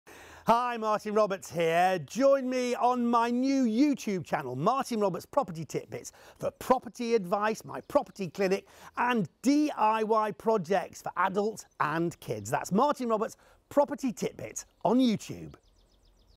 Martin Roberts Property Titbits Channel Trailer
Join Martin Roberts from Homes under the Hammer as he offers advice on Property and DIY. There's your chance to ask questions at the 'Property Clinic', Real life house renovations to follow, Behind the scenes from Homes under the Hammer, The Property Masterclass (where you can learn the fundementals of property investing and renovation), DIY disasters (with Wise Alek), DIY Hacks and DIY project ideas for Adults and Kids.